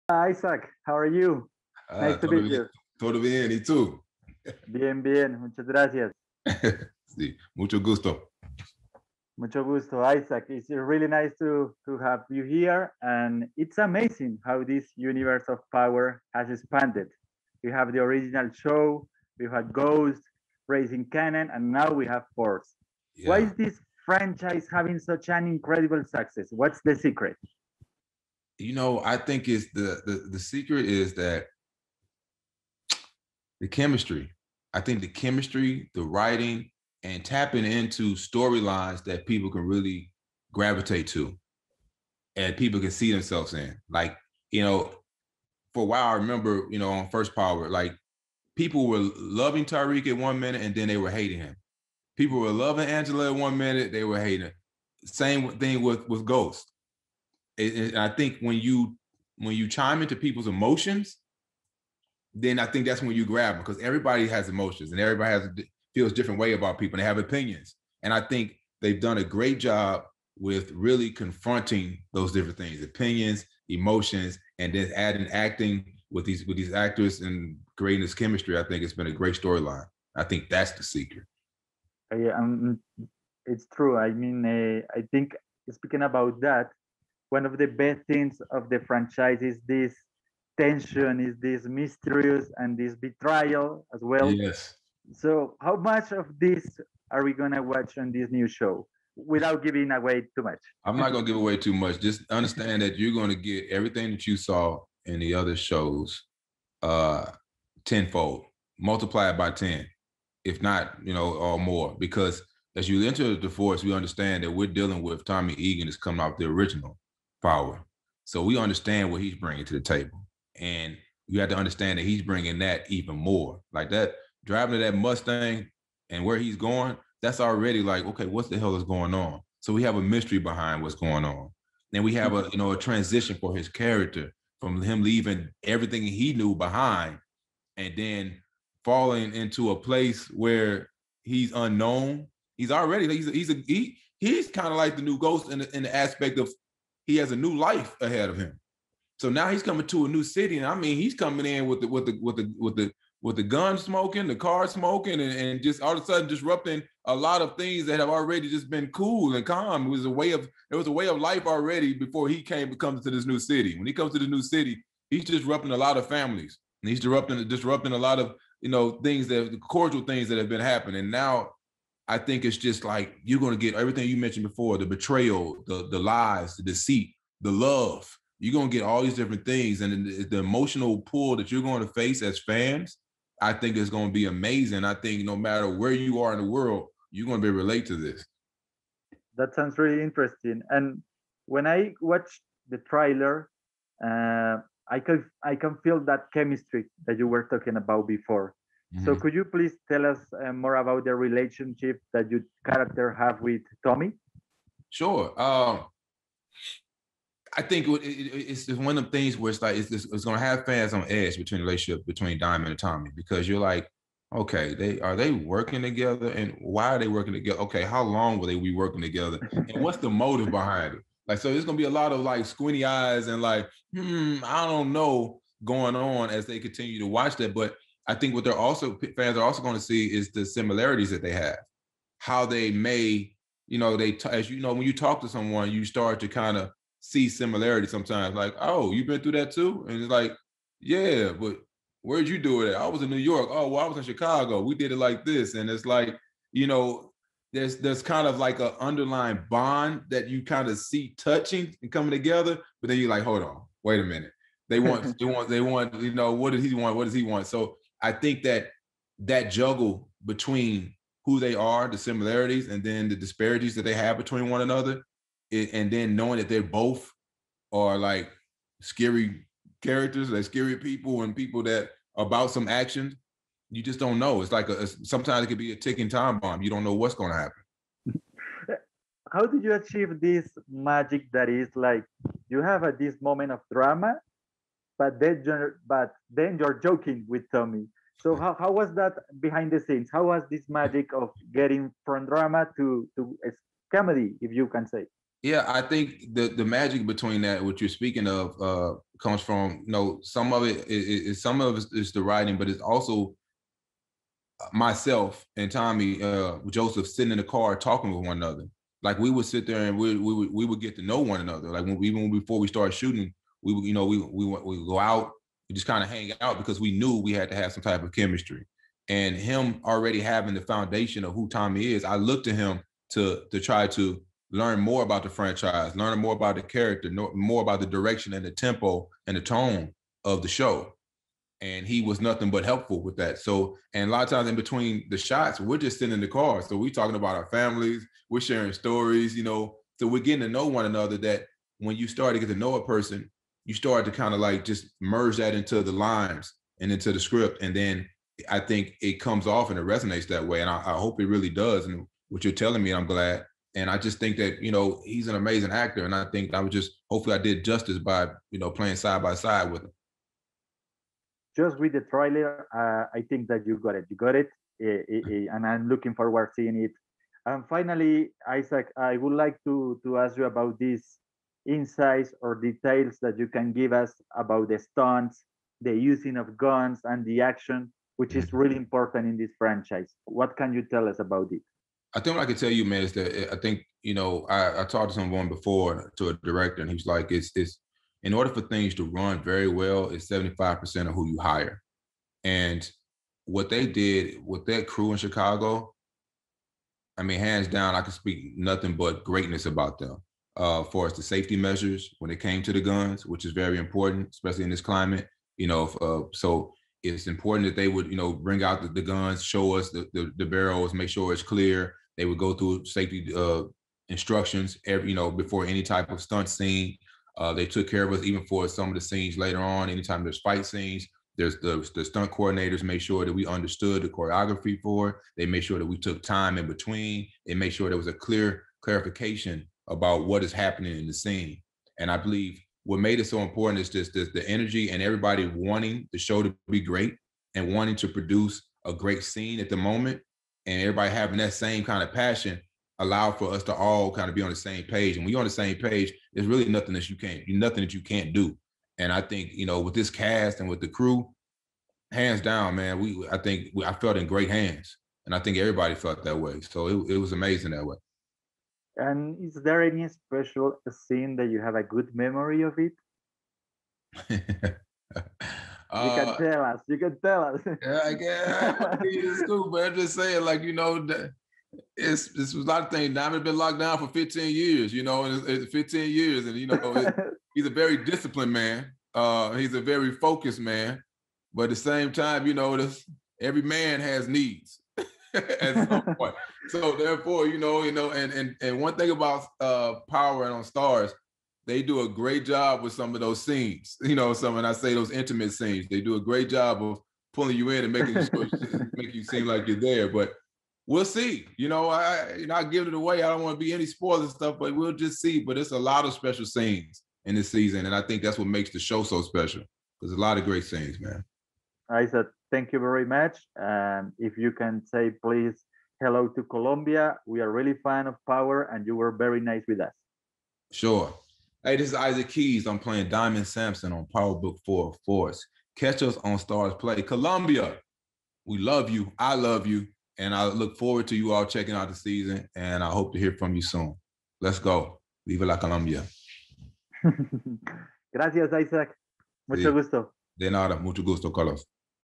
Entrevista Isaac Keys de Power Book IV: Force
Nuestro colega Carlos Londoño de @bandadelcomic realizó una gran entrevista para el canal a uno de los protagonistas de la serie Power Book IV: Force de Starzplay